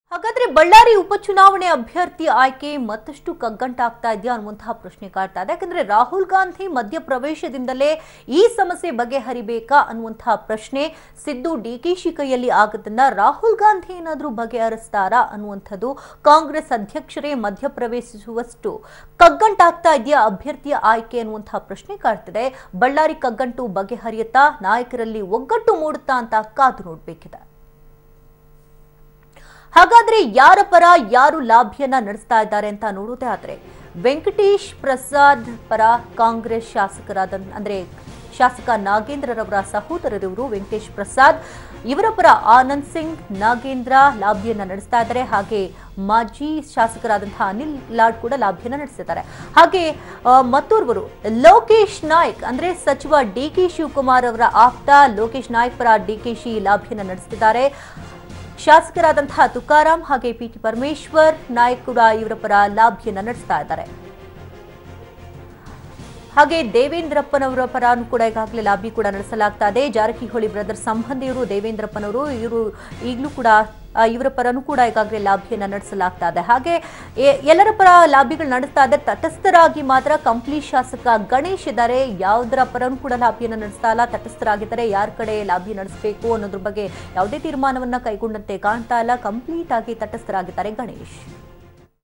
Congress Seeks Rahul Gandhi's Help; By-Poll Candidates To Be Announced Today
The Bellary Lok Sabha byelections are turning out to be a headache for Congress and Rahul Gandhi is likely to take a final call on the candidates.